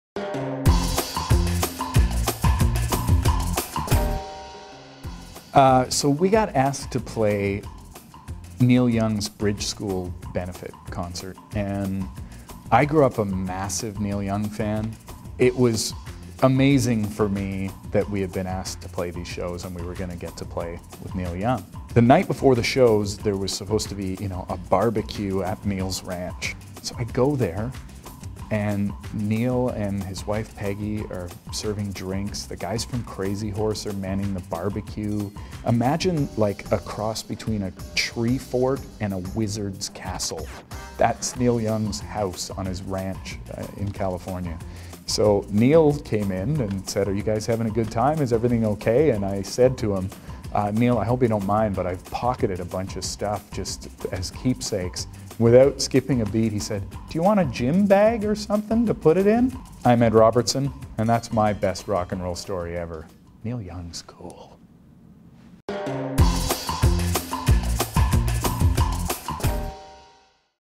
BEST STORY EVER: Ed Robertson Hangs With Neil Young
Ed Robertson of the Barenaked Ladies is a huge Neil Young fan. So when he got the chance to play Neil's Bridge School Benefit, he was happy to do it.

One thing he didn't necessarily expect: getting to hang out with Neil himself, and Crazyhorse to boot. But it happened... And Neil was a great host.